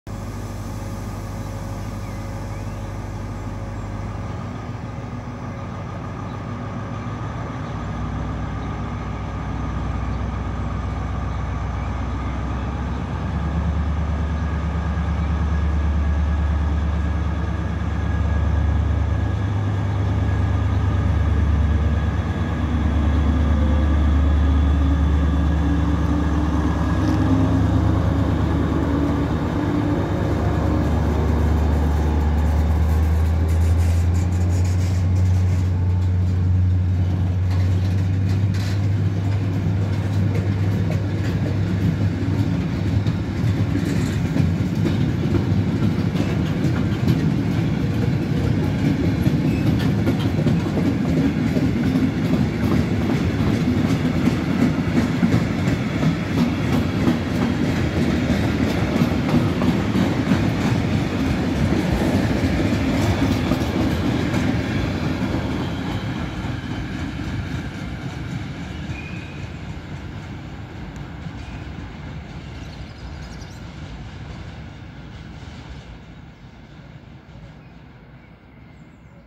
294 839 von Railion fährt mit einem Güterzug aus dem Bahnhof Guntershausen
Dir hat das Video gefallen? Dir hat das Video gefallen?